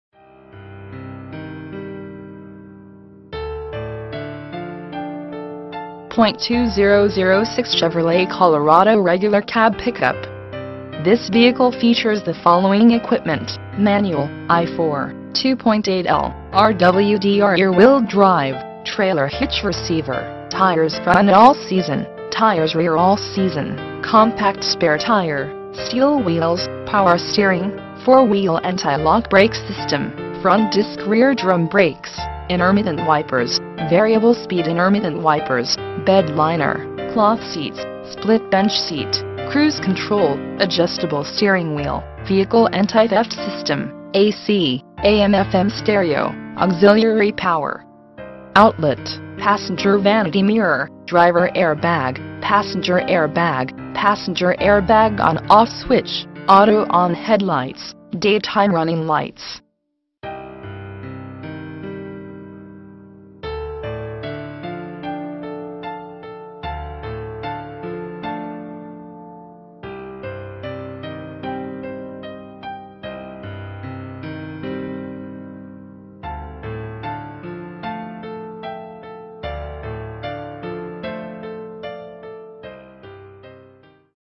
2006 Chevrolet Colorado - Classic Dealer Group
For Sale: 2006 Chevrolet Colorado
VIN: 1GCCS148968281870
Engine: I4 2.8L
Drivetrain: RWD
Transmission: Manual
Mileage: 84,437
Color: Summit White (exterior) Light Cashmere (interior)

Features:
2006 Chevrolet Colorado Regular Cab Pickup.